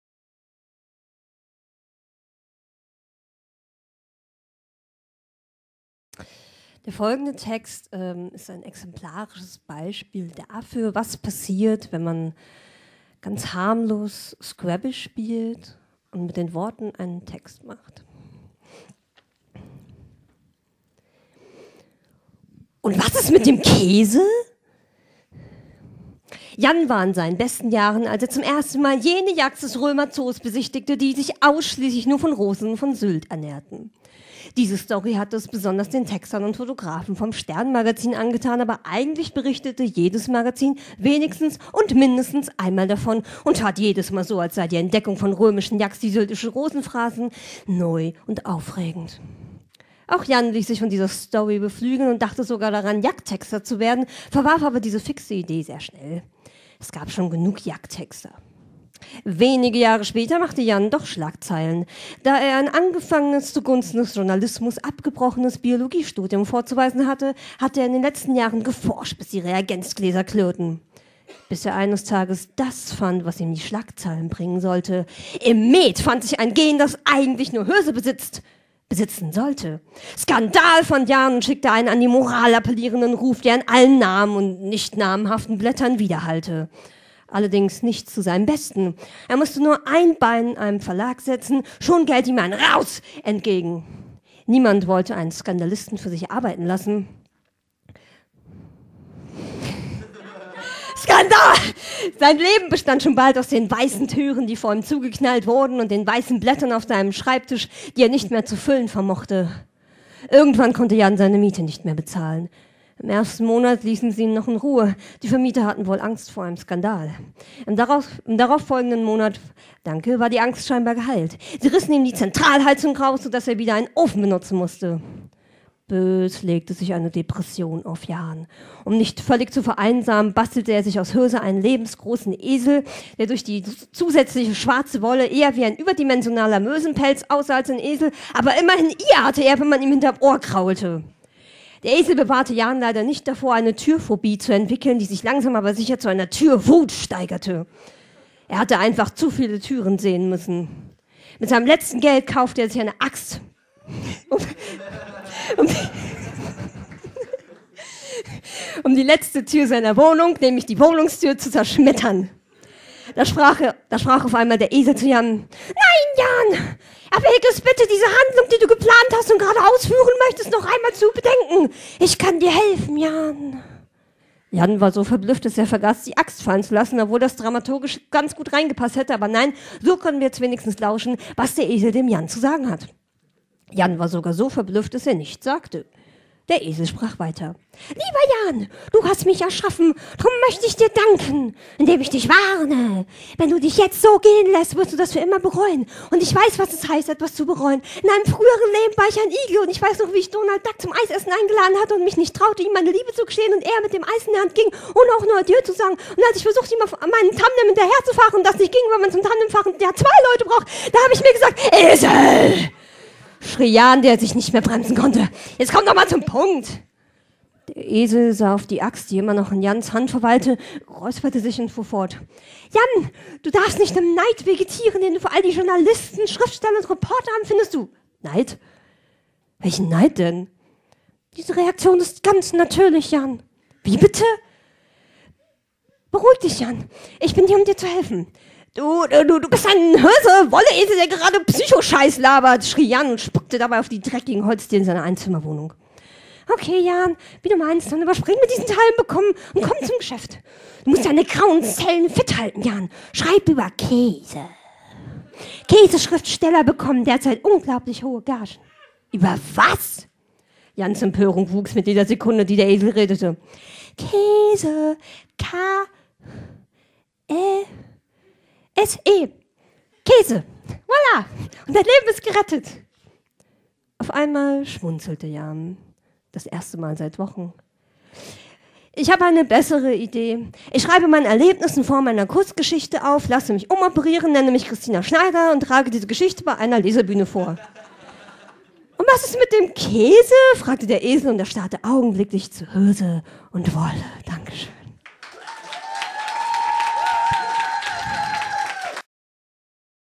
Und was ist mit dem Käse? - Christina Schneider
Ein Text von Christina Schneider, gelesen bei der Lauschgift - Lesebühne im Kurt-Lade-Klub...